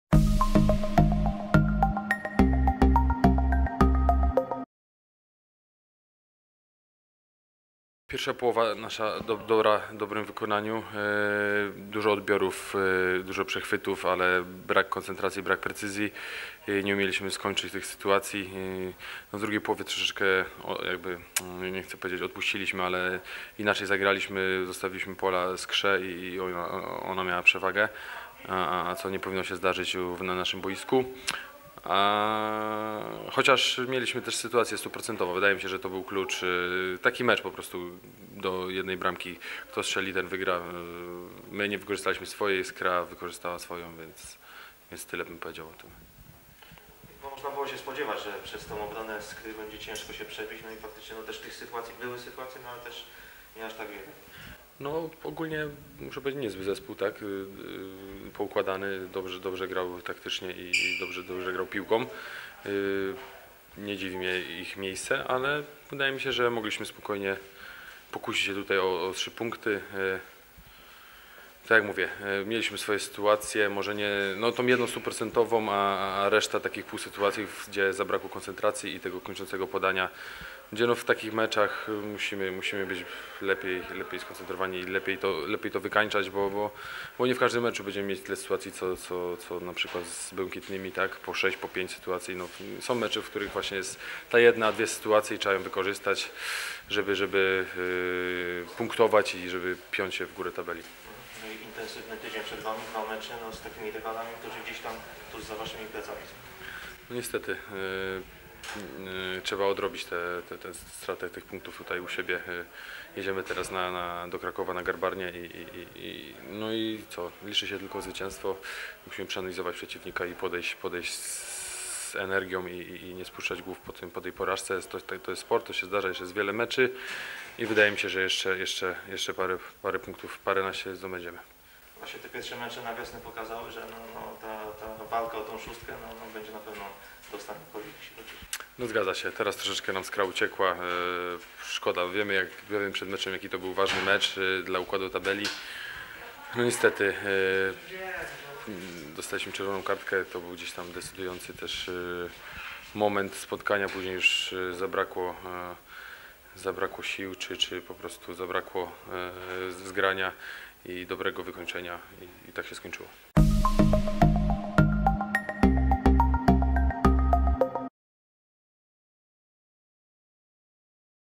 Tomasz Hołota po meczu ze Skrą Częstochowa: "Zabrakło koncentracji i precyzji".
- W drugiej połowie zostawiliśmy Skrze dużo miejsca i to rywale mieli przewagę, co nie powinno się wydarzyć na naszym obiekcie. (...) To był mecz do jednej bramki - tak skomentował boiskowe wydarzenia, w rywalizacji ze Skrą Częstochowa, Tomasz Hołota, zawodnik "Trójkolorowych". Przypomnijmy, że w ostatnim ligowym spotkaniu, rozgrywanym na Stadionie Miejskim przy ul. Łódzkiej 19-29 w Kaliszu, KKS uległ przyjezdnym 1:0. Cała wypowiedź kakaesiaka dostępna w naszej klubowej telewizji.